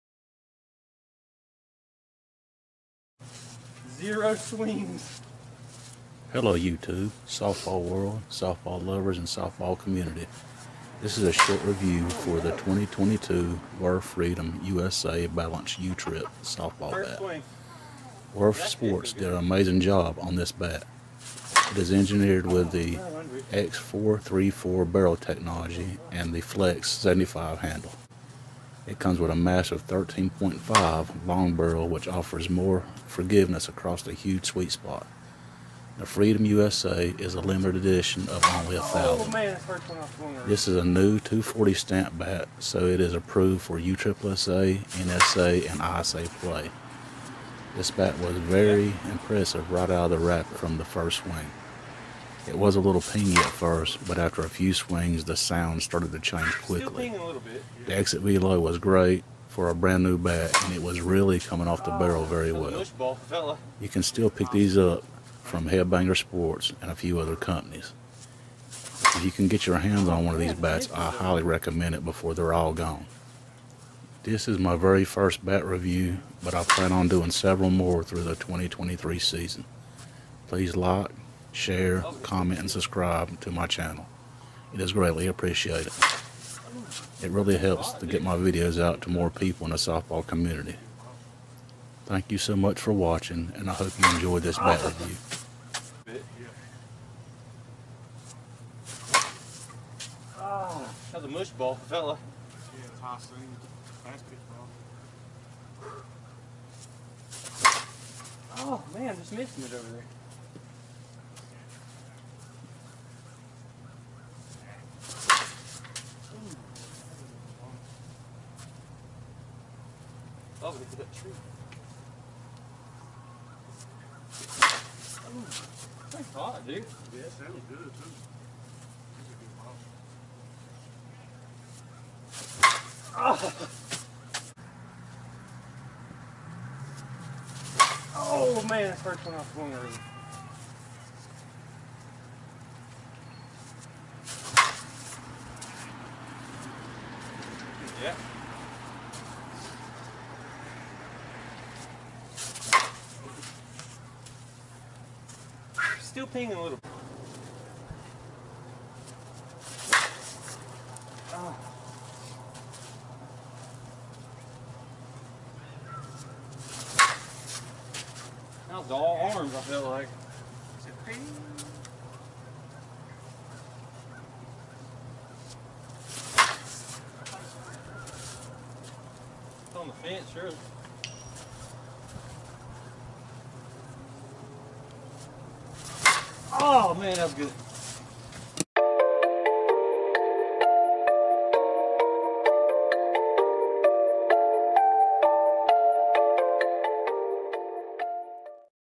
2022 Worth Freedom USA Balanced Slowpitch Bat Review ⚾💪 #2023
2022 Worth Freedom USA Balanced Slowpitch Bat Review ⚾💪 #2023
Let Freedom Ring with this 2022 Worth Freedom Balanced USSSA Slowpitch Softball Bat! This hitstick was designed for people who love the red, white & blue and crush bombs.

It's engineered with our X434 barrel technology which utilizes exclusive carbon fiber material to create a more responsive barrel with insane pop. This Freedom bat is equipped with a massive 13.5" barrel which offers more forgiveness across it's huge sweet spot. In addition, the Flex 75 handle offers the perfect combination of flex and feel so you can swing with confidence every pitch. This bat is available in three different weights, each with a balanced end load for ultimate bat control.

 If you play in a USSSA, NSA or ISA league, this Freedom Slowpitch Bat will transform your game and turn you into a beast on the field.

The 2022 Worth Freedom USSSA Slowpitch Softball Bat features the standard 2 1/4 inch barrel diameter, 13.5" barrel length, balanced swing weighting, and is legal for play in all USSSA, NSA, and ISA Slowpitch Softball Leagues and Tournaments.

Freedom Limited Edition of 1000 between all companies.
13.5 inch Barrel Length.
2 1/4 Inch Barrel Diameter.
Two-Piece Composite Slowpitch Softball Bat.
Balanced Swing Weighting.
X434 Composite Barrel.
Flex 75 Handle.
Huge Ready out of the Wrapper Barrel.
One Year Manufacturer Warranty.
Legal For Play in all USSSA, NSA, and ISA Sanctioned Leagues and Tournaments.